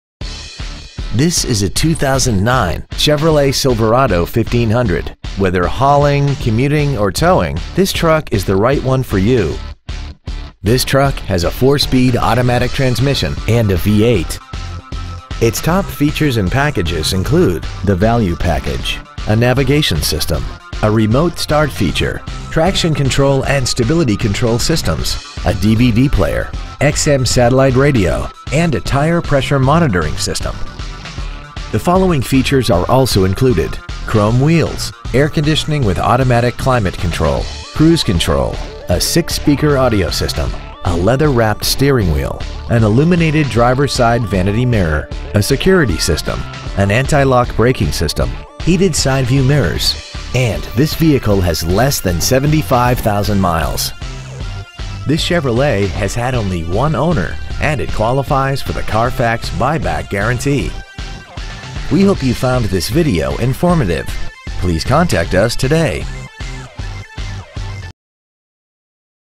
2009 Chevrolet Silverado 1500 Merced CA 95340
We have been honored to serve the Merced CA area , we promise that your experience at our dealership will exceed your expectations !

 Year : 2009
 Make : Chevrolet
 Model : Silverado 1500

 Trans . : Automatic
 Exterior : Graystone Metallic
 Miles : 74,363
 Interior : Ebony